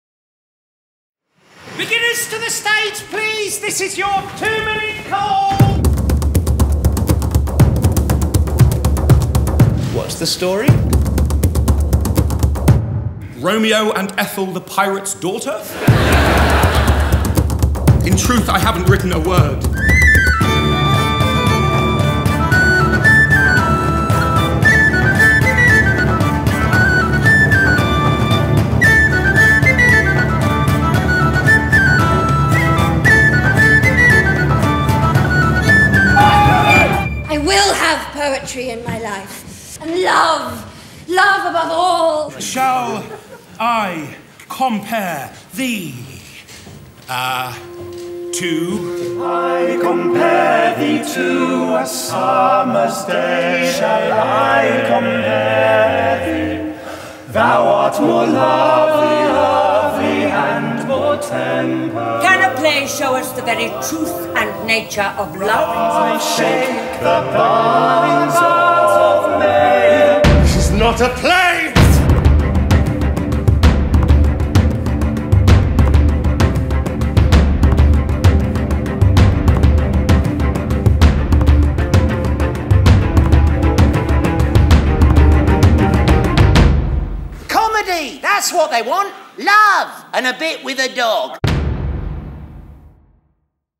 Shakespeare in Love on stage - trailer
Adapted from the award-winning film, Shakespeare in Love is now playing at the Noel Coward Theatre in London's West End